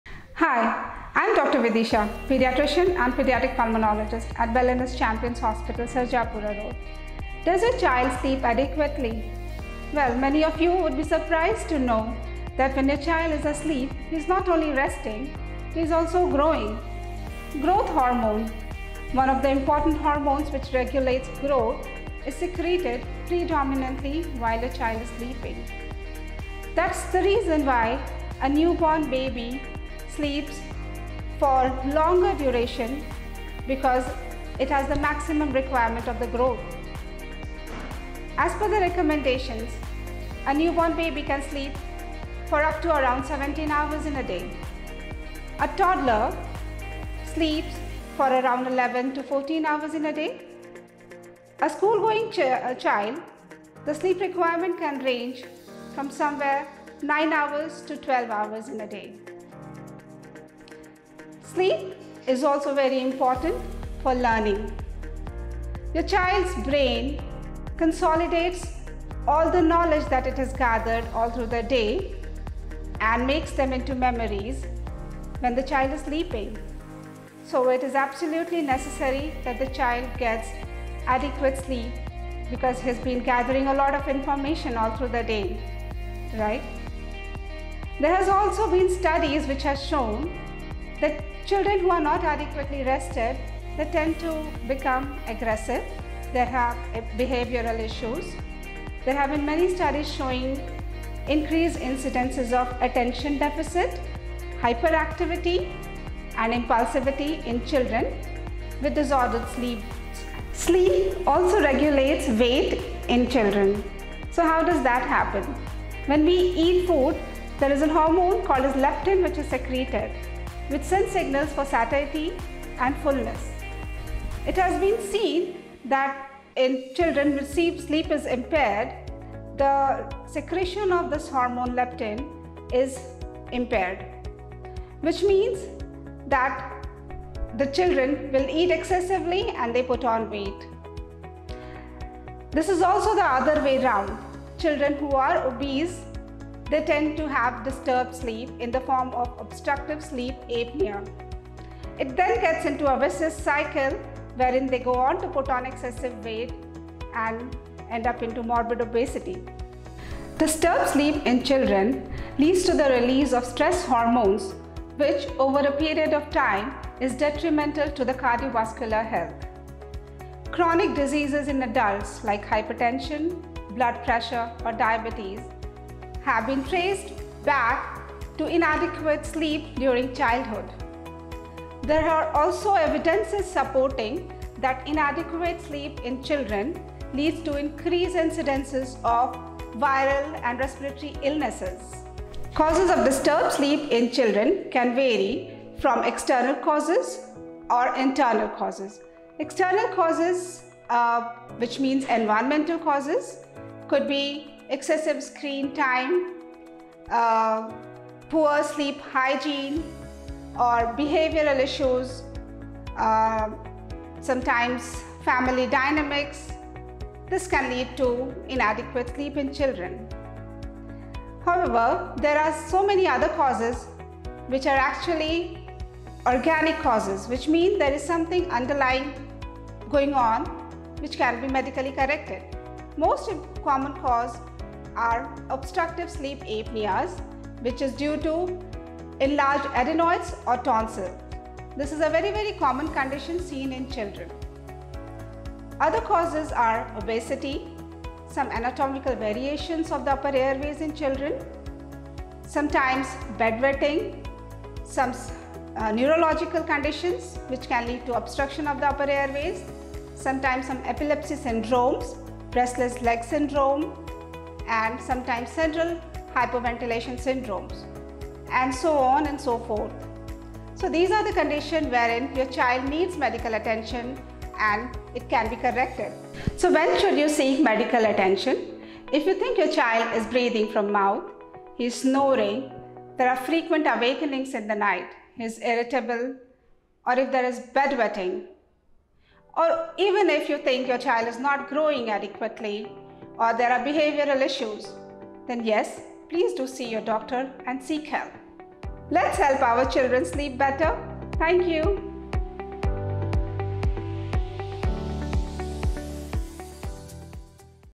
Dr Vidisha (Consultant Pediatrics) on importance of Sleep for Children
In this informative video, Dr. Vidisha, a consultant pediatrics at Belenus Champion Hospital, discusses the importance of sleep for children's health and well-being.

Dr. Vidisha highlights the various benefits of adequate sleep for children, including improved cognitive function, enhanced immune system, and better emotional regulation. She emphasizes that sleep is an essential building block for a child's growth and development, and that getting enough sleep is crucial for children to thrive and reach their full potential.

Furthermore, Dr. Vidisha provides practical tips and advice for parents on how to establish healthy sleep habits for their children, such as maintaining a consistent bedtime routine, limiting screen time before bed, and creating a comfortable and conducive sleep environment.

Through her expertise and engaging delivery, Dr. Vidisha makes a compelling case for the importance of sleep and provides valuable insights for parents seeking to ensure their children get the restful and restorative sleep they need to grow and thrive.